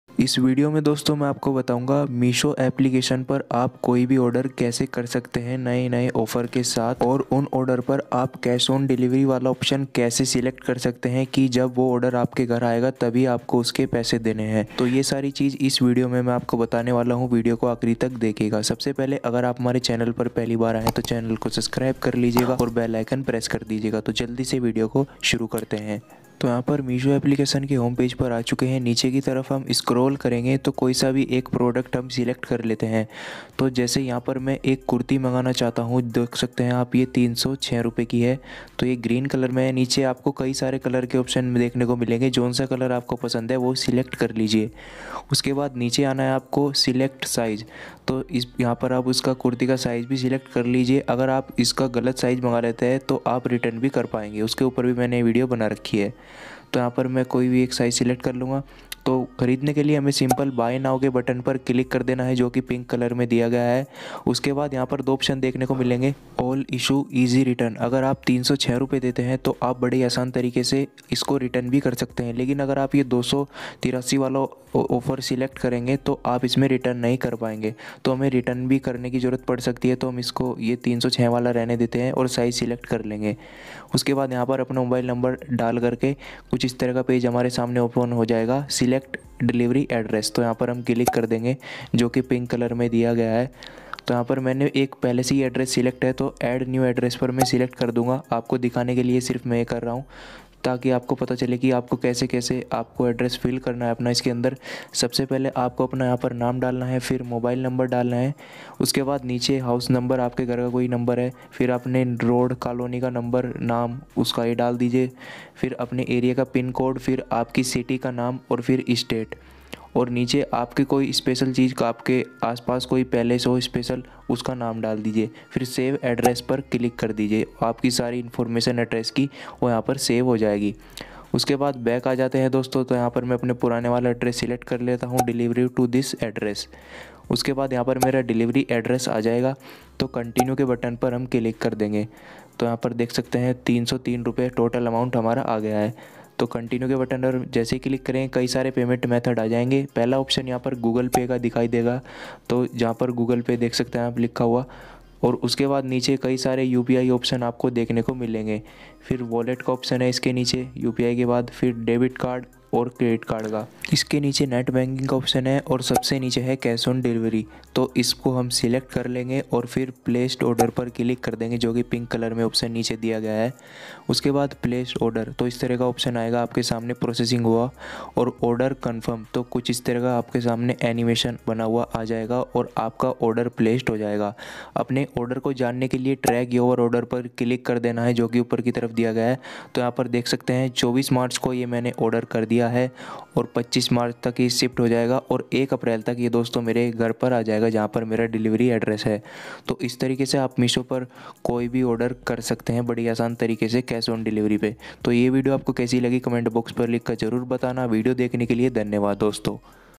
Meesho app par order kaise kare || How to order on meesho app || meesho se order kaise kare ?
Hello friends in this video I will tell you how you can order on meesho app.